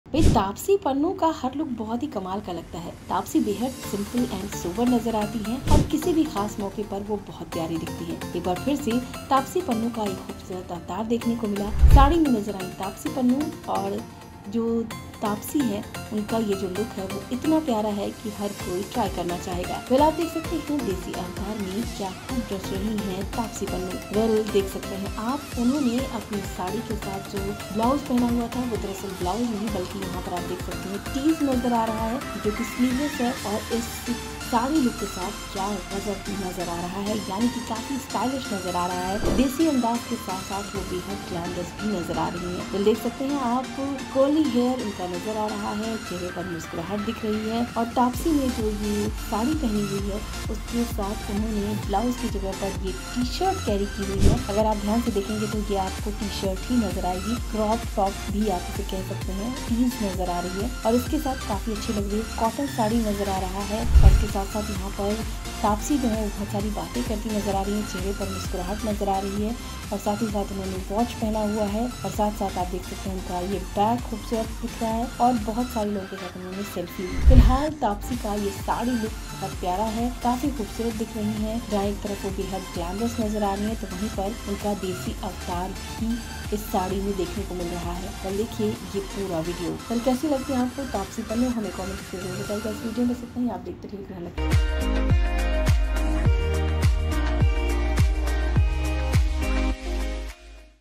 Taapsee Pannu का ये कॉटन साड़ी लुक जिसे आप भी कर सकते हैं कैरी | Grehlakshmi गृहलक्ष्मी
Taapsee Pannu’s  elegant yet simple cotton saree look redefines grace.Watch video.

तापसी पन्नू सिंपल एंड सोबर लुक में बेहद ही कमाल की लगती हैं। उनका साड़ी लुक दिखा बेहद खास। तापसी कॉटन साड़ी में खूब जमीं। देखिए वीडियो